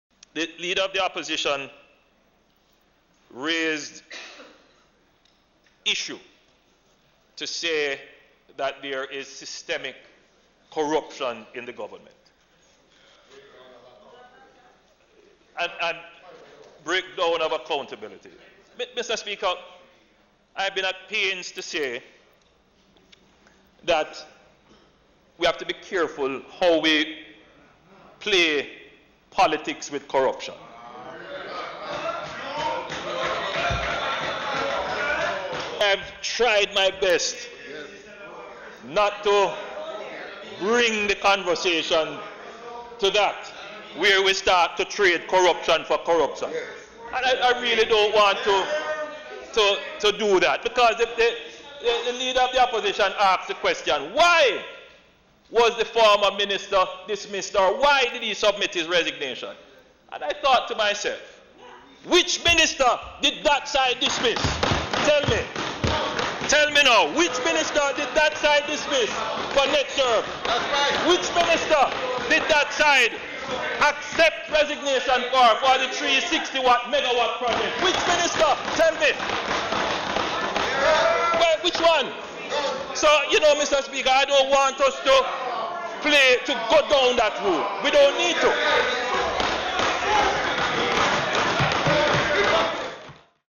PM Andrew Holness dismisses claims of corruption in his administration.
Leader of Opposition Dr. Peter Phillips and the PNP has repeatedly tried to paint a picture that the Andrew Holness administration is on that masked with corruption. However, neither Dr. Phillips or his party has never dealt with corruption allegations in the PNP.